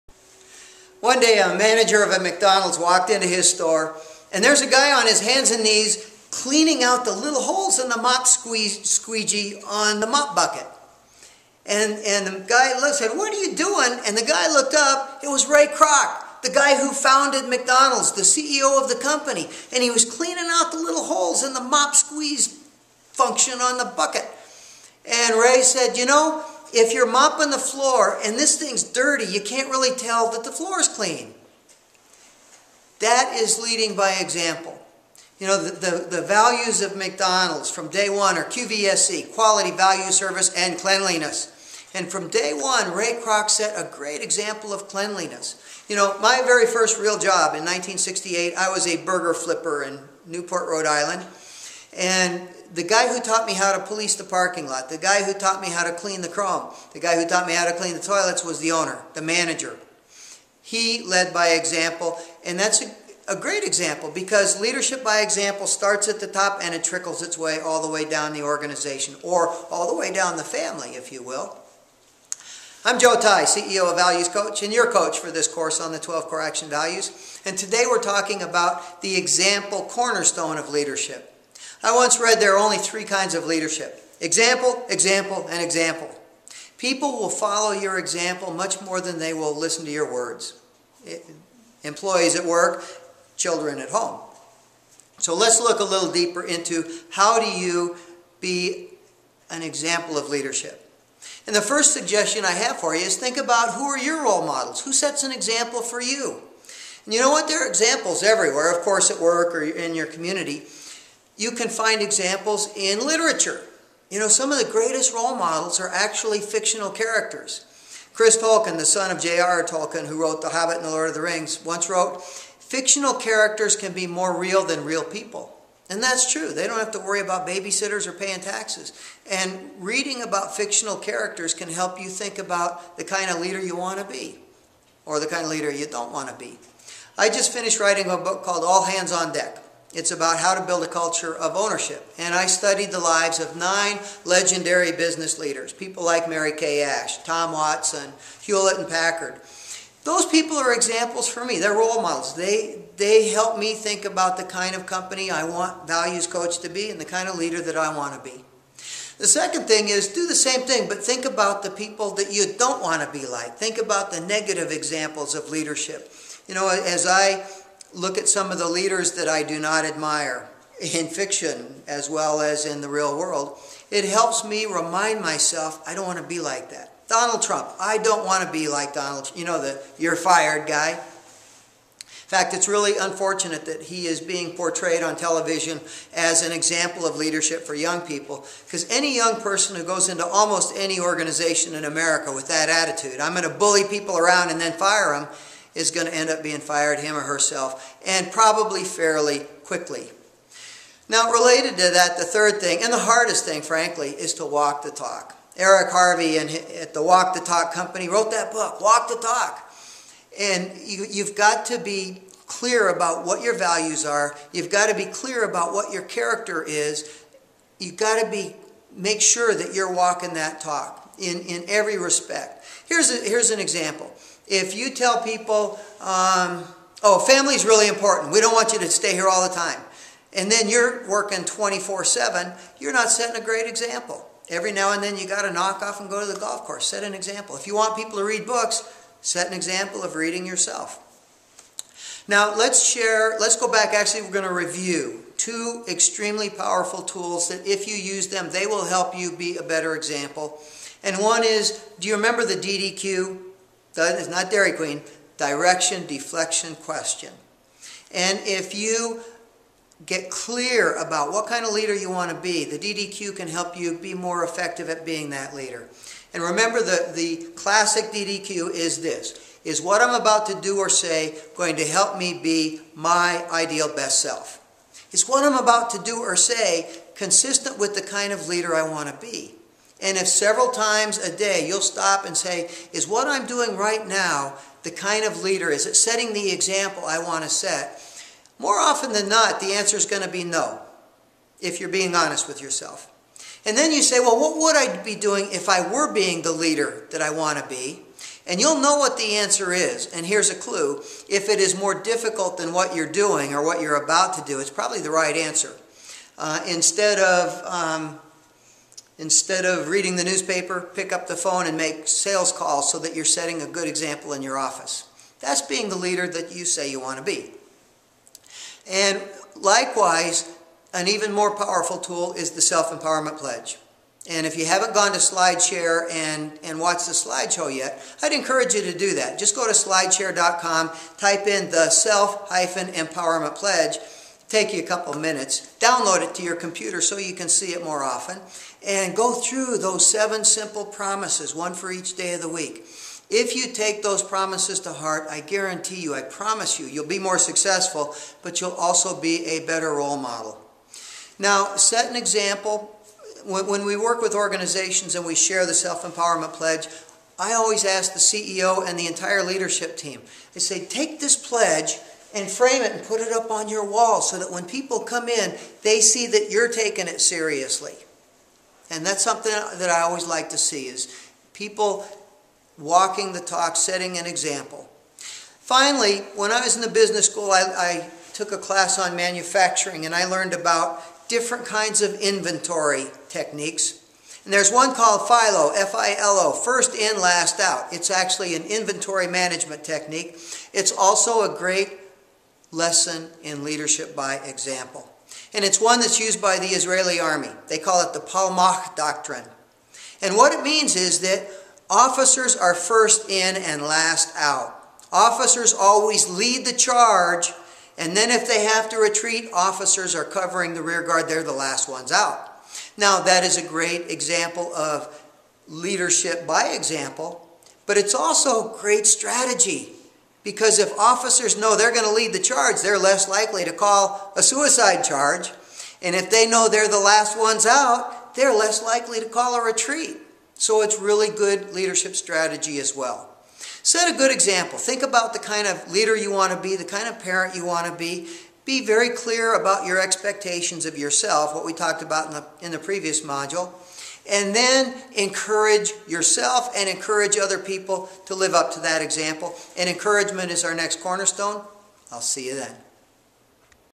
Introductory video to the Example Cornerstone of Leadership
Introductory video for the Example Cornerstone of Core Action Value #12, Leadership, presented by Values Coach CEO Joe Tye